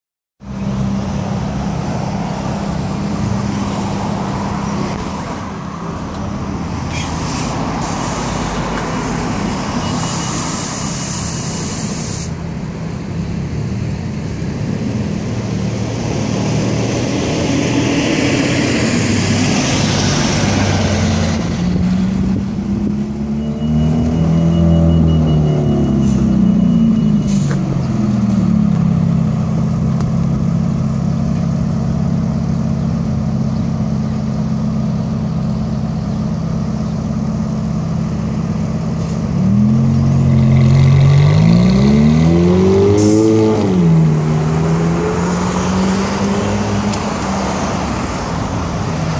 Ferrari F430 Spider V8 Supercar
Drive off visual w the sublime Spider. Leaving the car parts store, the Ferrari F430 Spider awaits the green light.